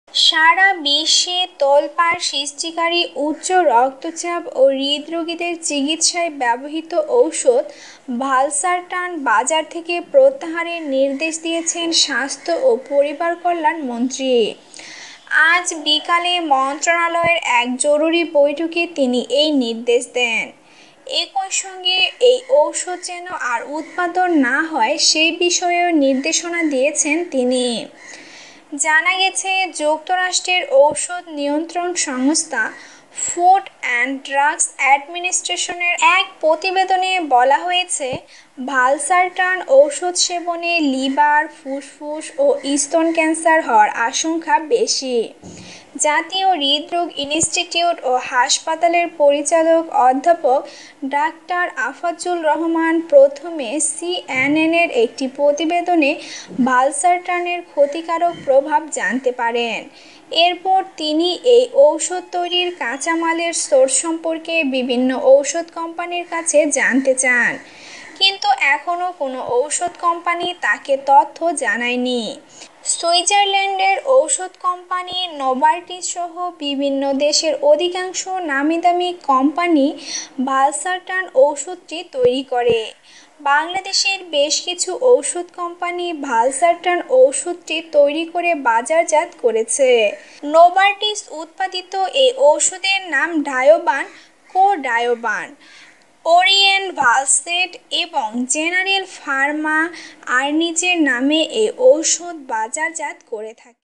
ভালসারটান ওষুধ প্রত্যাহারের নির্দেশ!
ভালসারটান ওষুধ প্রত্যাহারের নির্দেশ!! কারন না জানলে বিপদে পড়ে যাবেন??

সারা বিশ্বে তোলপাড় সৃষ্টিকারী উচ্চ রক্তচাপ ও হৃদরোগীদের চিকিৎসায় ব্যবহৃত ওষুধ ভালসারটান বাজার থেকে প্রত্যাহারের নির্দেশ দিয়েছেন স্বাস্থ্য ও পরিবারকল্যাণমন্ত্রী মোহাম্মদ নাসিম। রোববার বিকালে মন্ত্রণালয়ে এক জরুরি বৈঠকে তিনি এ নির্দেশ দেন। একই সঙ্গে এ ওষুধ আর যেন উৎপাদন না হয় সে বিষয়েও নির্দেশনা দিয়েছেন তিনি।